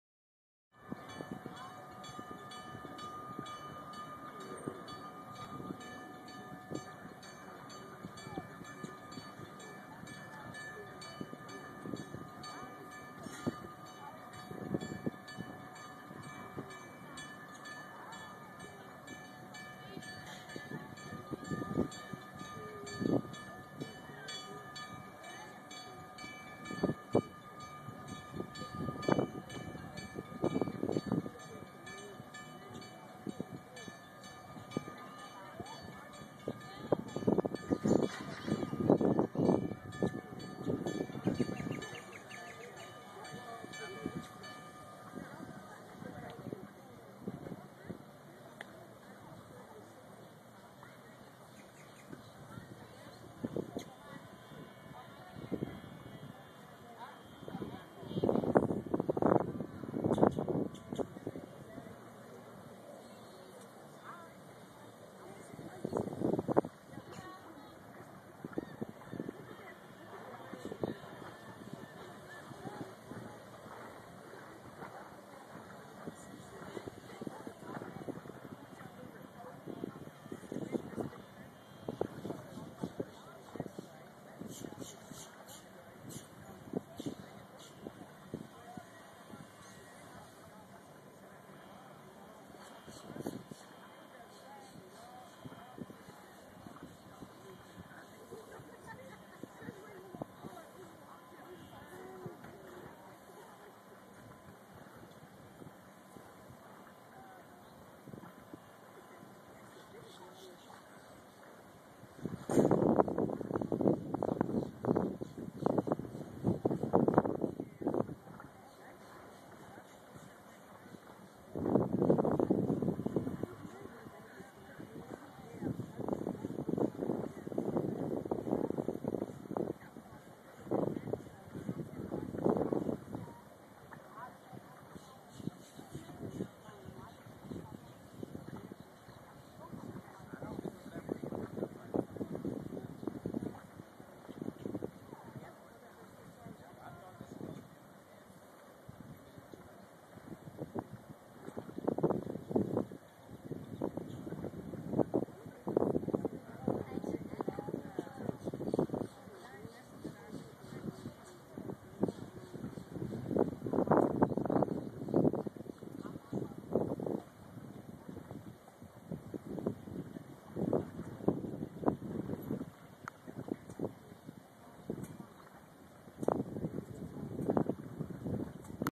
Woods Memorial Bridge, Beaufort, SC opening up
Did you ever see a swing-bridge in operation?   The Woods Memorial Bridge connecting downtown Beaufort, SC to its Sea Islands opens to let a passing sailboat thru.